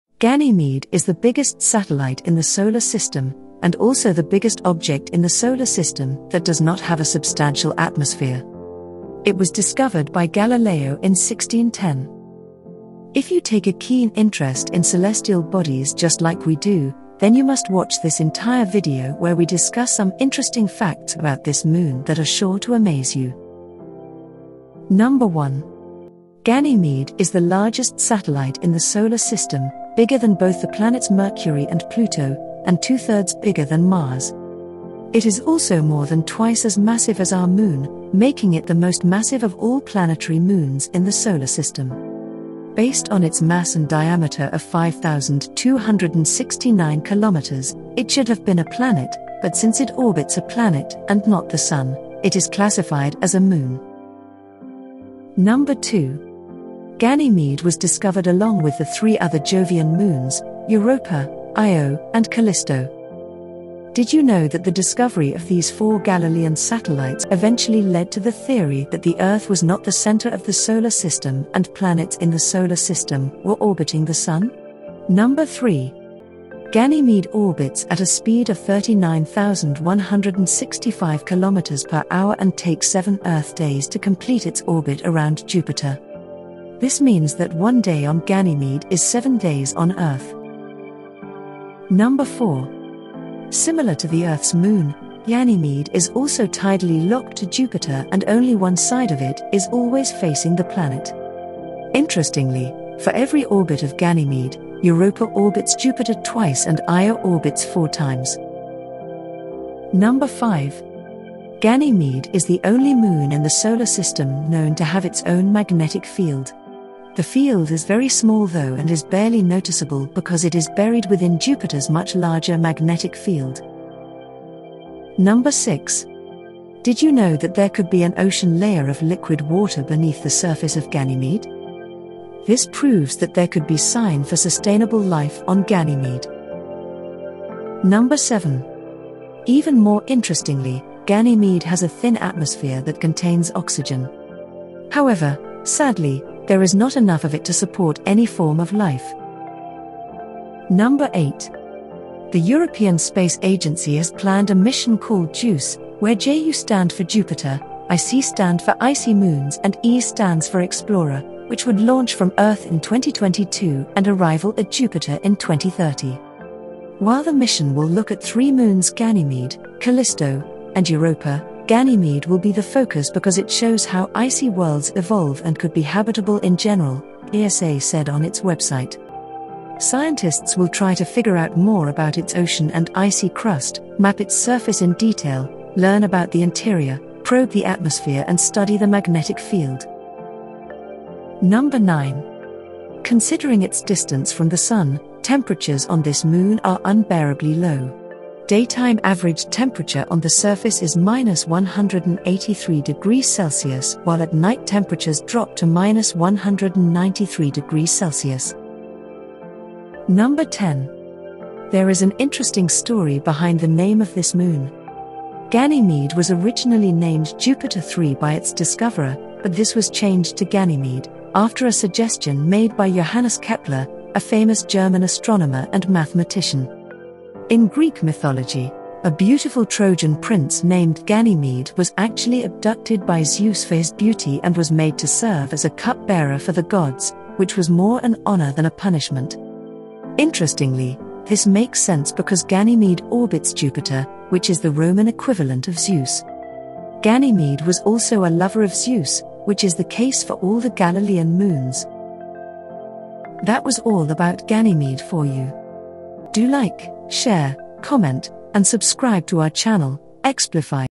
Ganymede - Biggest Satellite In Our Solar System
Ganymede is the biggest satellite in the solar system, and also the biggest object in the solar system that does not have a substantial atmosphere. Discovered by Galileo in January of the year 1610 along with Io, Callisto and Europa, it was the motion of these bodies around Jupiter that led to the realization that everything in the solar system was orbiting the Sun, and that Earth was therefore not the center of the Universe. If you take a keen interest in celestial bodies just like we do, then you must watch this entire video where we discuss some interesting facts about this moon that is sure to amaze you. So let’s begin!

Thumbnail image source (Creative Commons 2.0 Attribution)